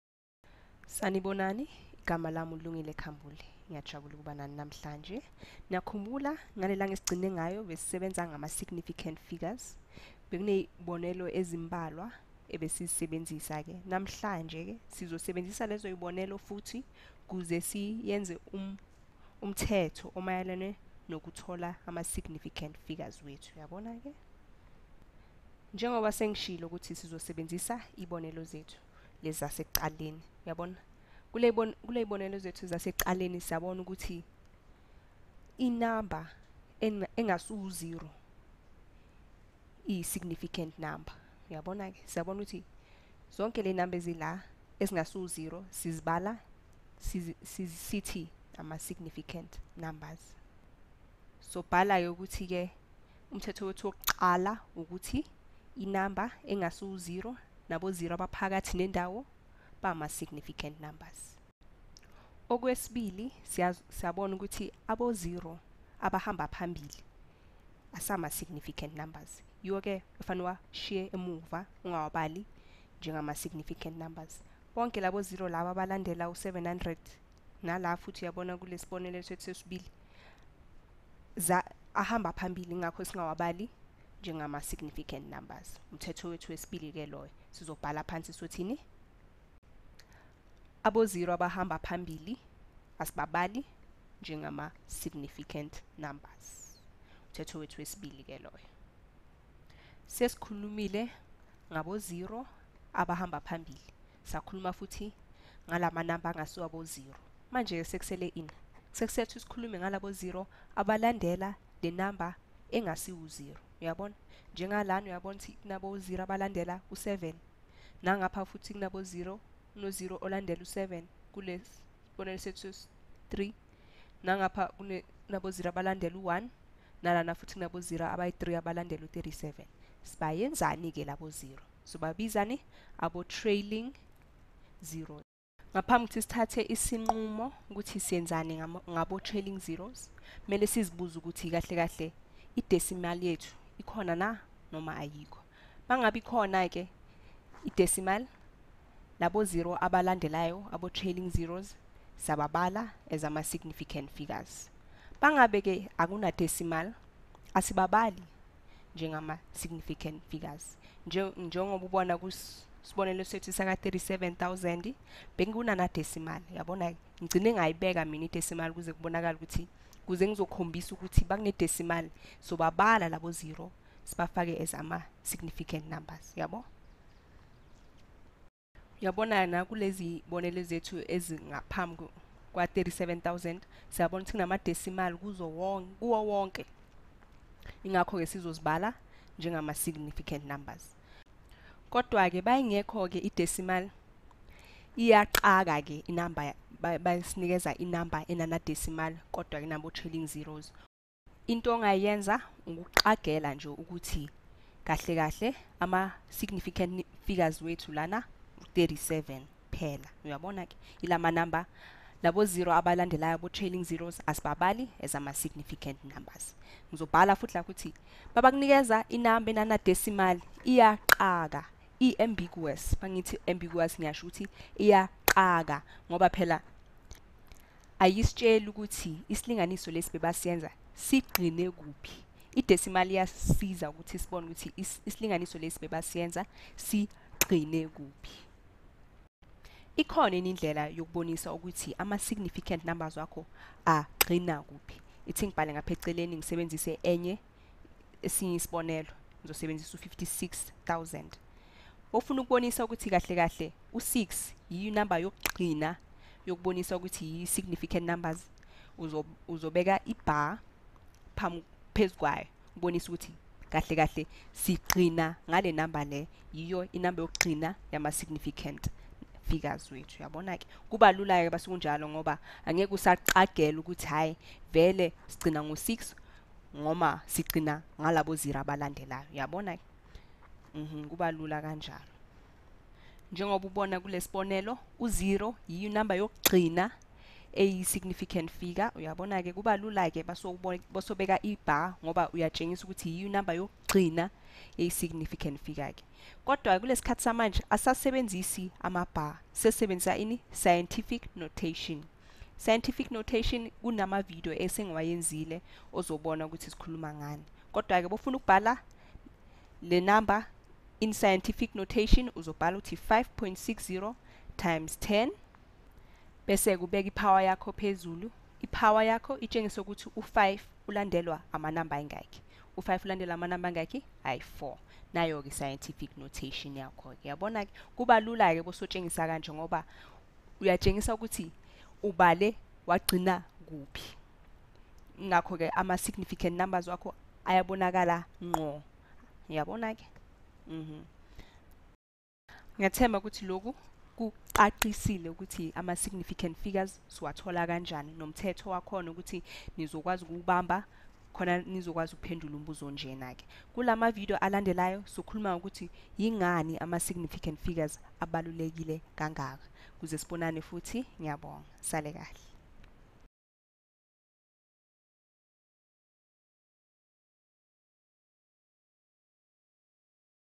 More on significant figures
This original Khan Academy video was translated into isiZulu by Lungile Khambule. The translation project was made possible by ClickMaths